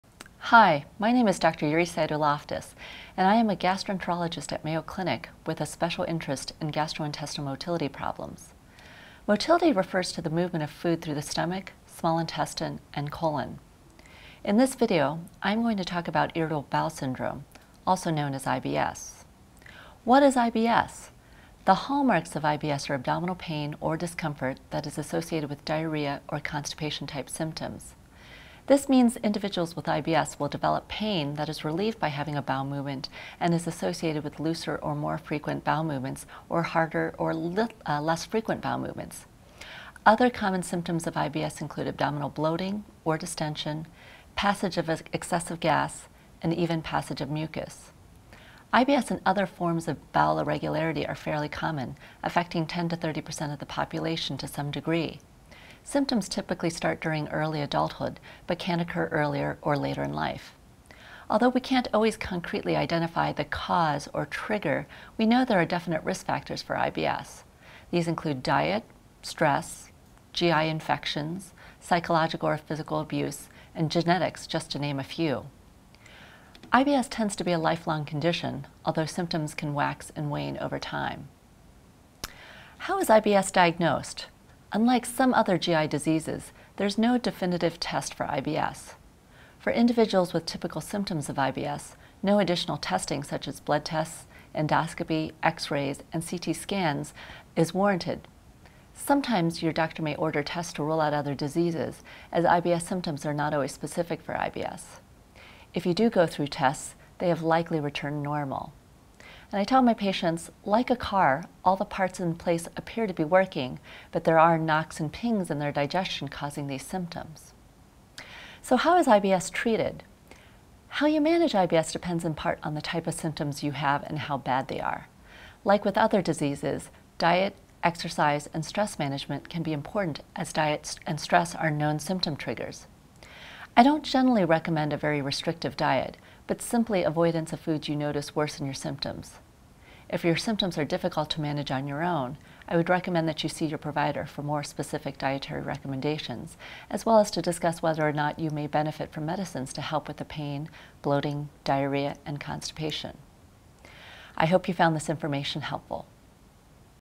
IBS - Yuri Saito Loftus, M.D.
Yuri Saito Loftus, M.D., a Gastroenterologist at the Mayo Clinic with a special interest in gastrointestinal motility problems talks about the diagnosis and treatment of Irritable bowel syndrome, also known as IBS.